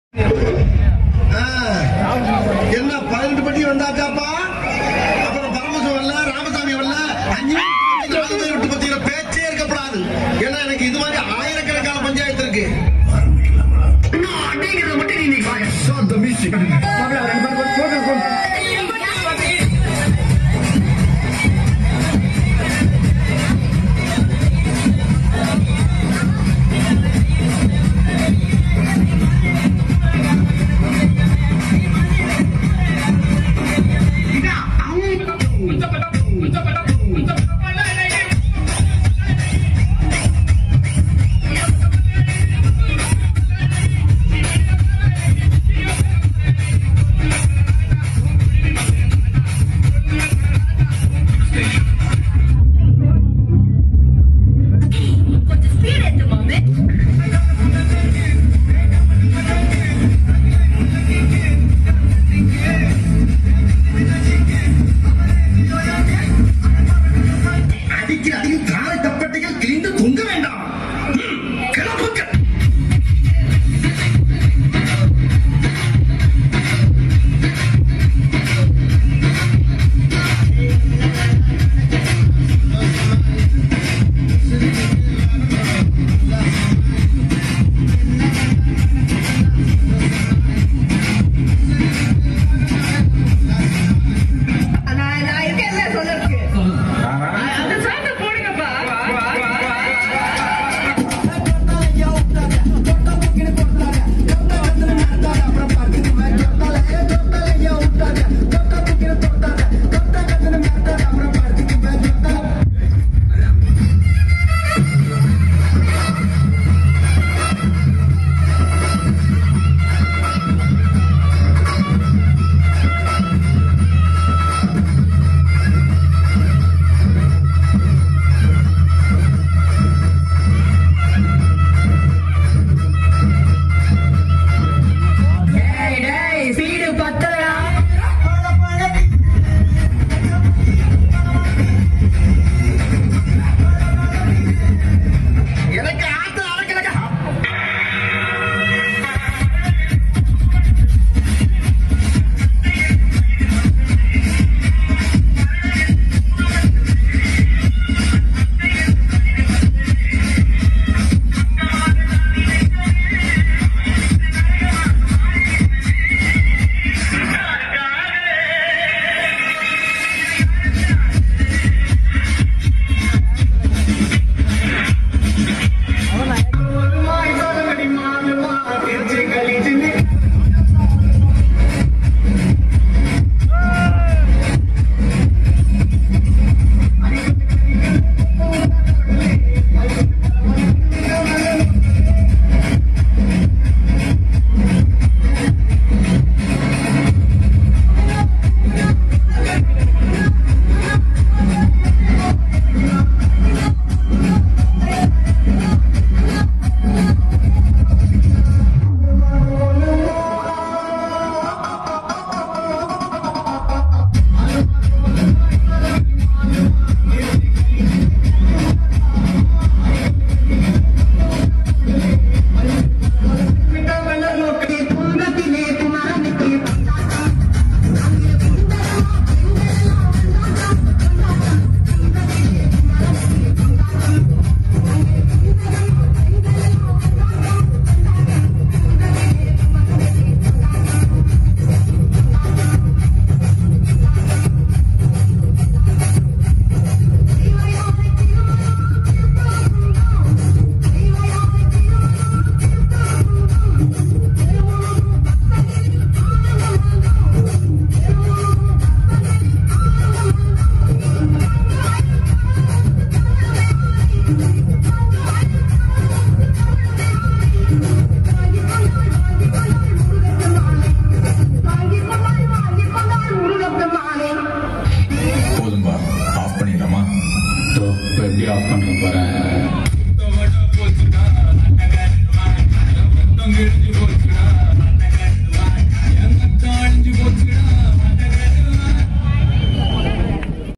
Tamil mixing song // Hnuc her sec school arianayagipuram village thenkasi dt
dance performance at school anualday function Tamil mixing song // Hnuc her sec school arianayagipuram village thenkasi dt